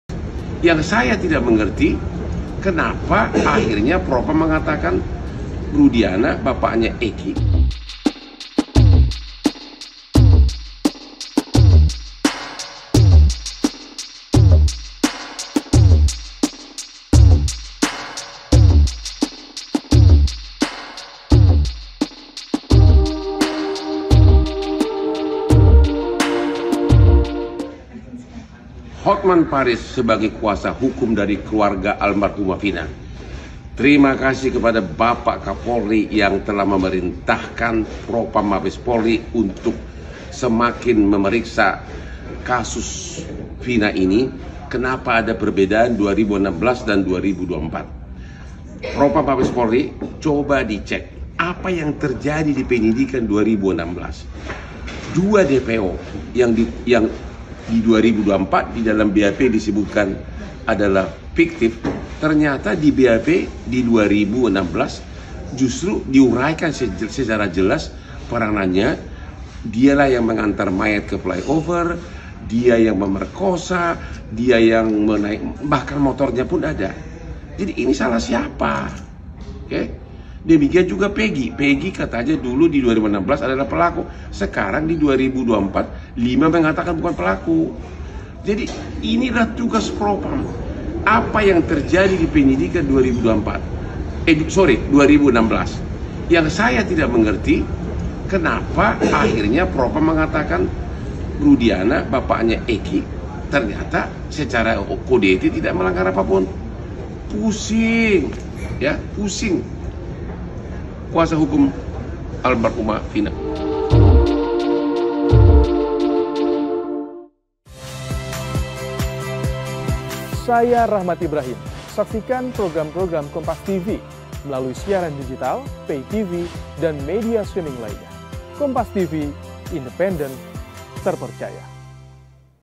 Pusing soal Kasus Vina Cirebon, Hotman Singgung Iptu Rudiana hingga BAP 2016
JAKARTA, KOMPAS.TV - Kuasa Hukum Keluarga Vina, Hotman Paris mengaku tidak mengerti mengapa akhirnya Propam mengatakan bahwa Ayah Korban Eky, Iptu Rudiana tidak melanggar kode etik.

"Saya tidak mengerti kenapa akhirnya Propam mengatakan Rudiana bapaknya Eky ternyata secara kode etik tidak melanggar apapun, pusing," ungkap Kuasa Hukum Keluarga Vina, Hotman Paris pada Selasa, (25/6/2024) di Akun Instagram pribadinya @hotmanparisofficial.

Hotman juga sempat menyinggung soal BAP kasus pembunuhan Vina-Eky Cirebon 2016 silam.